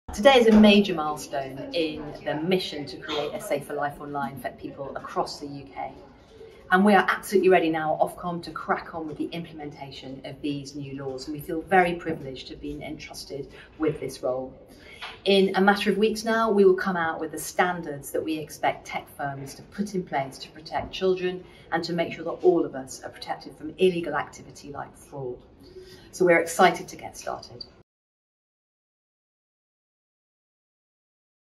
Online Safety Bill passed its final stage in parliament
Today is a major milestone in the mission to create a safer life online for people across the UK.

Ofcom Chief Executive, Dame Melanie Dawes, welcomes the Online Safety Bill passing its final stage in Parliament today.